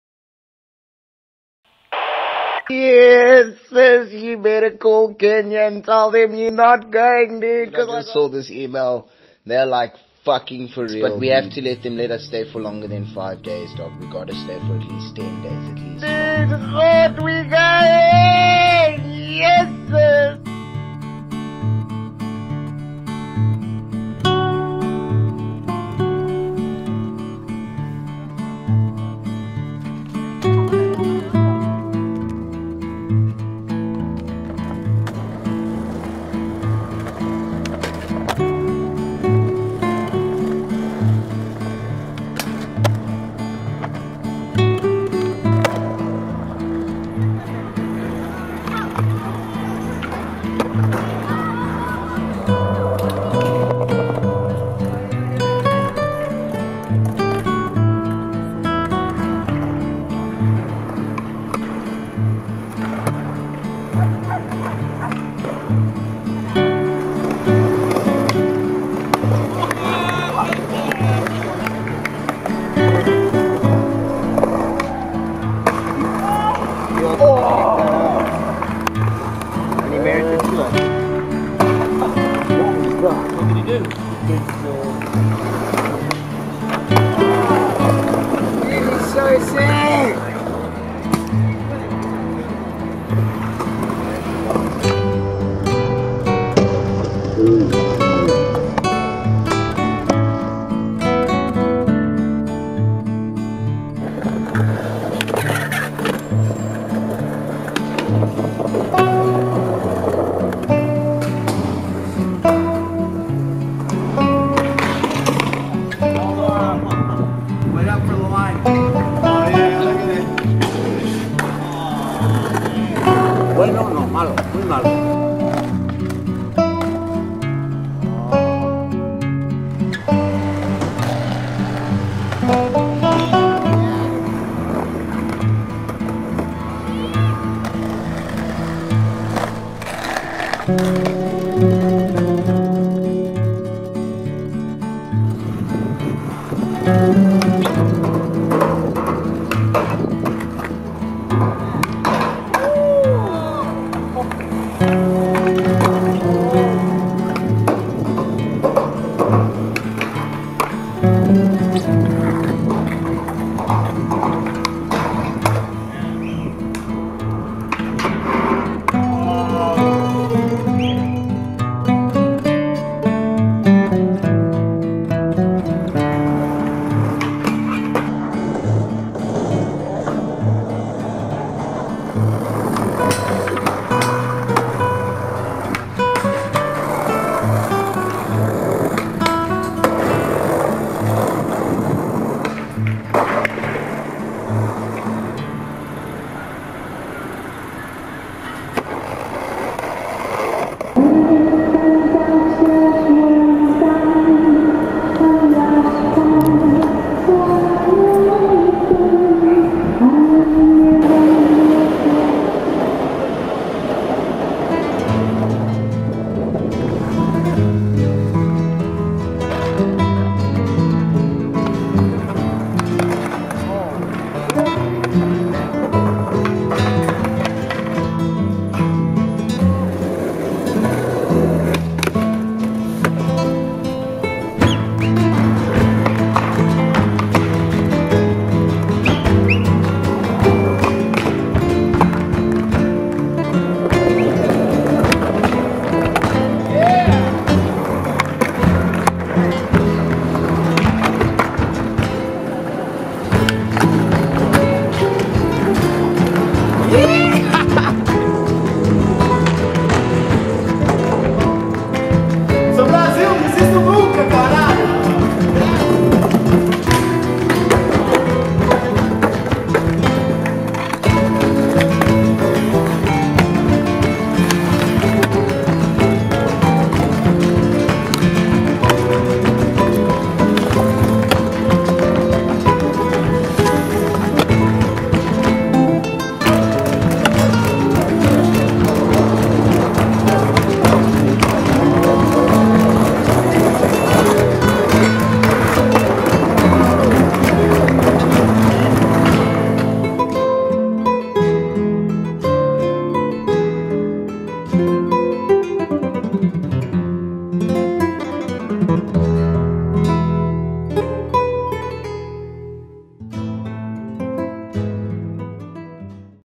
City Dwellers Vol.3 - Josh 'Spice' Chisholm in Barcelona
We headed to Barcelona for a few days and cruised the streets with Joshua Chisholm and friends from around the globe. Featuring Jordan Maxham, Ben Walters, Sean Malto, Simon Hallberg, Simon Karlsson, Alex Lekinhoo, Joubert van Staden